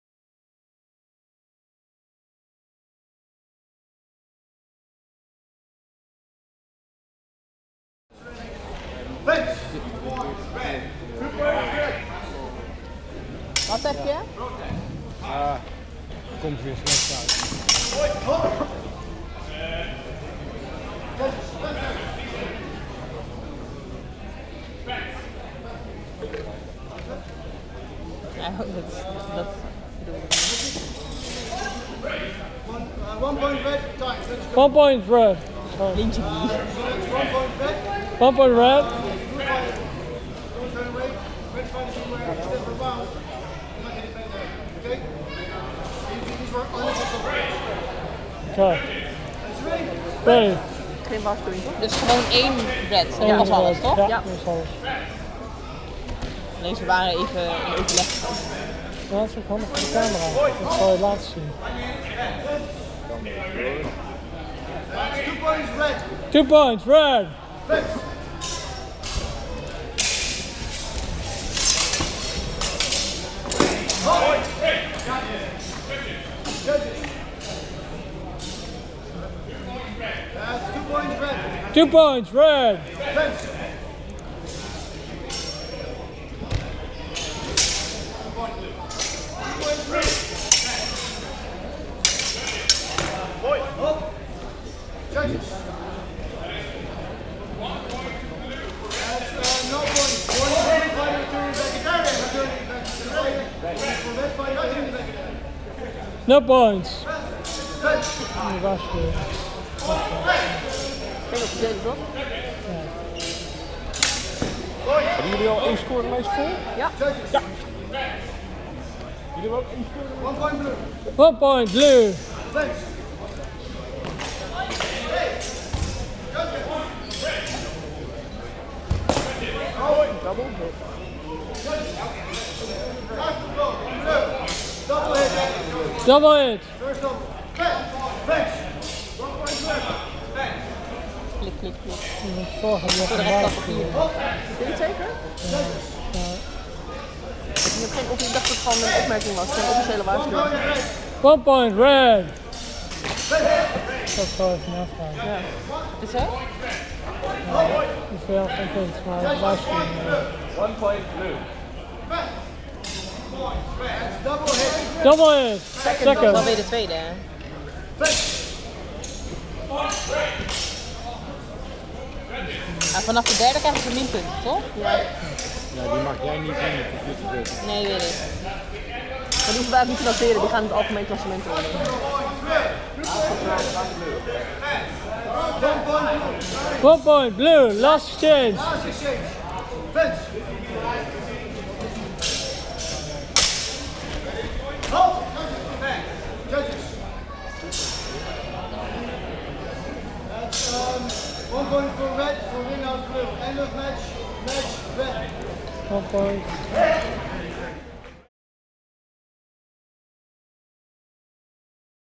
Poule 3 - Frederick Switsers vs Yannick de Smet
Dutch Lions Cup - Poule 3

Dutch Lions Cup - 27 August 2016 - Utrecht, The Netherlands
First International H.E.M.A. Open in The Netherlands
Hosted by Zwaard & Steen